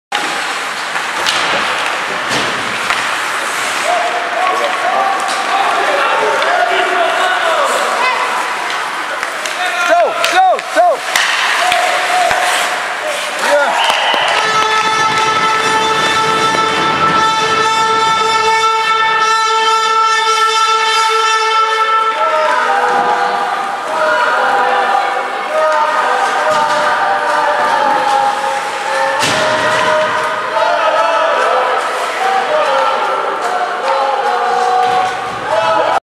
1 bramka Macieja Kostyła w meczu Żaków S na Bytomiu, 19 01 2013
1 bramka Macieja Kostyła w meczu Żaków Starszych, UKS MOSM Bytom - Tyskie Lwy MOSM Tychy, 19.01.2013